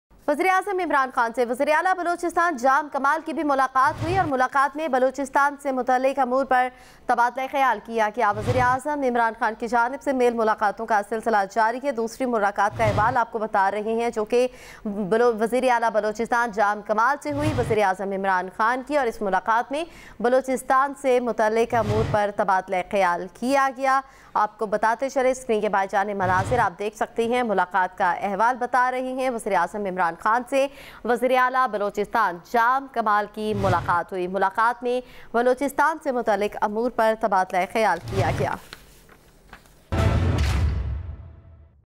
CapitalTV; PM Imran Khan meets CM Balochistan Jam Kamal
Prime Minister of Pakistan Imran Khan on Monday has met Chief Minister Balochistan Jam Kamal, Balochistan related affairs were discussed during the meeting